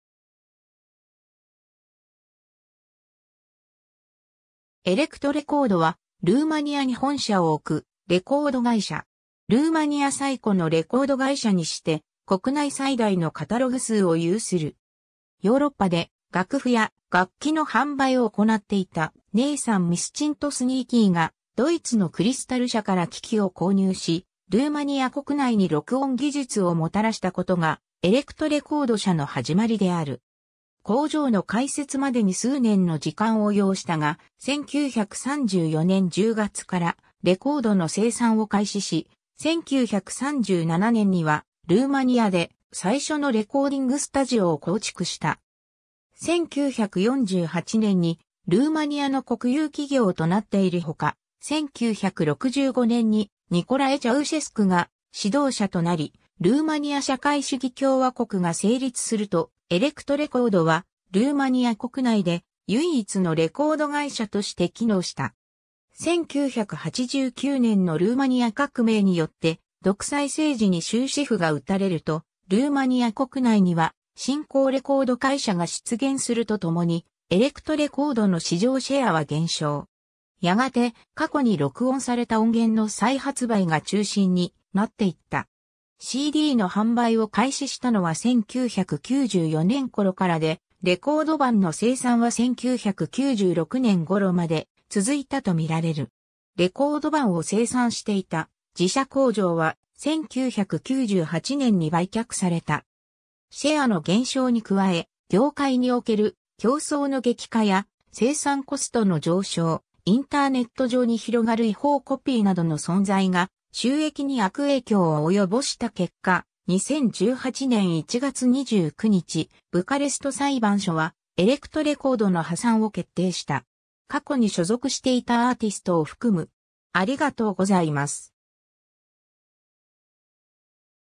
エレクトレコード
エレクトレコード（ルーマニア語: Electrecord）は、ルーマニアに本社を置くレコード会社。
ルーマニア最古のレコード会社にして、国内最大のカタログ数を有する。
 ヨーロッパで楽譜や楽器の販売を行っていたNathan Mischonznikiが、ドイツのKrystall社から機器を購入し、ルーマニア国内に録音技術を齎したことがエレクトレコード社の始まりである。
 工場の開設までに数年の時間を要したが、1934年10月からレコードの生産を開始し、1937年には、ルーマニアで最初のレコーディングスタジオを構築した（それまでの録音作業はウィーンやベルリンへ通って行っていた）。
 1948年にルーマニアの国有企業となっているほか、1965年にニコラエ・チャウシェスクが指導者となり、ルーマニア社会主義共和国が成立すると、エレクトレコードはルーマニア国内で唯一のレコード会社として機能した。
 1989年のルーマニア革命によって独裁政治に終止符が打たれると、ルーマニア国内には新興レコード会社が出現するとともに、エレクトレコードの市場シェアは減少。
やがて、過去に録音された音源の再発売が中心になっていった。
 CDの販売を開始したのは1994年頃からで、レコード盤の生産は1996年頃まで続いたとみられる。
レコード盤を生産していた自社工場は、1998年に売却された。
 シェアの減少に加え、業界における競争の激化や、生産コストの上昇、インターネット上に広がる違法コピーなどの存在が収益に悪影響を及ぼした結果、2018年1月29日、ブカレスト裁判所はエレクトレコードの破産を決定した。
 ※過去に所属していたアーティストを含む。